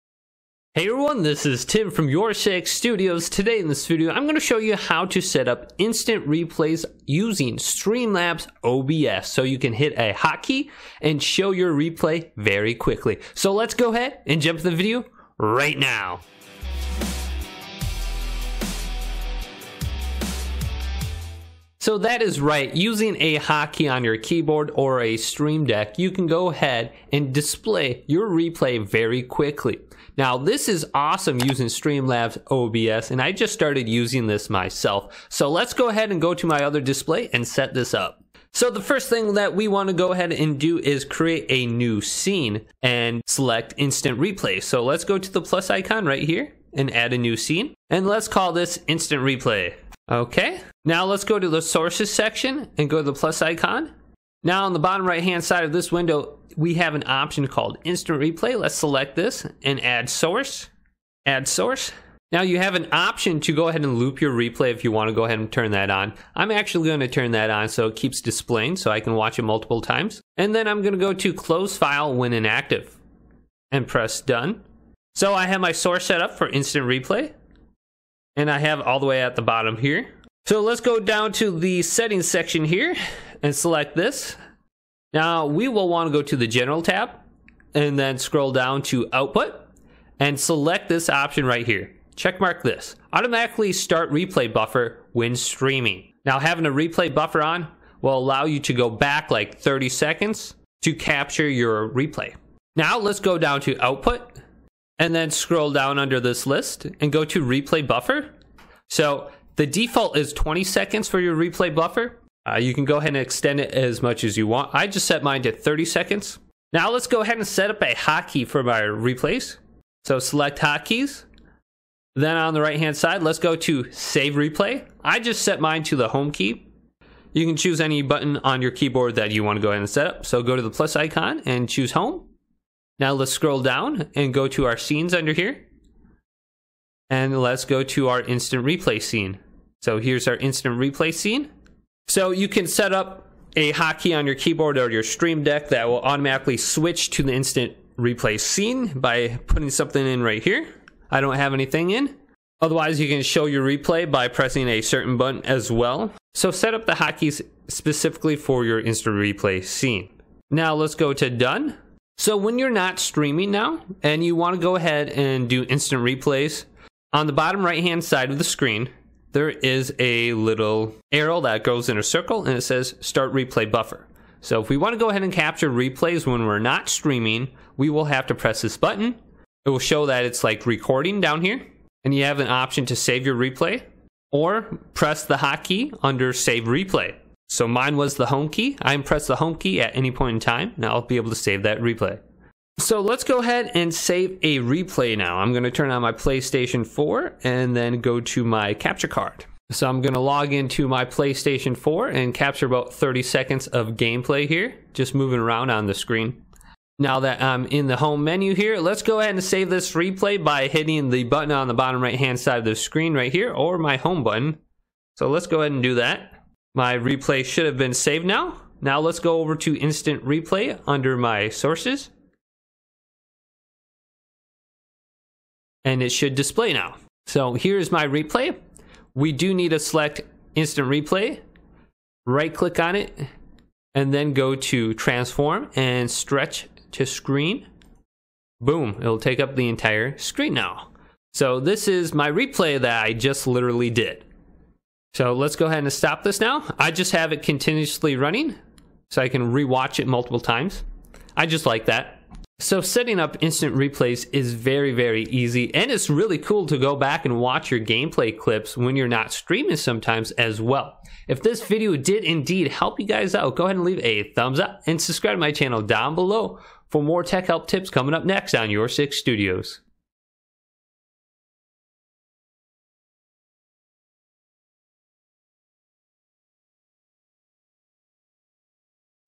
How to Setup Instant Replay using Stream Labs OBS While Live Streaming (Fast Method!)
Do you want to know how to Setup Instant Replay using stream labs obs while your streaming or just recording video? This is very easy to do, you can  setup hotkeys that will switch to the save replay scene quickly or setup a hotkey to save the last 30 seconds of gameplay. This is very cool and it works similar to how the streamdeck hot key for twitch works, but allows you to save the replay to your PC. Also, you can automatically setup replays when your streaming. There is a instant replay scene that you will have to setup and be aware of the sound mixer for the replay.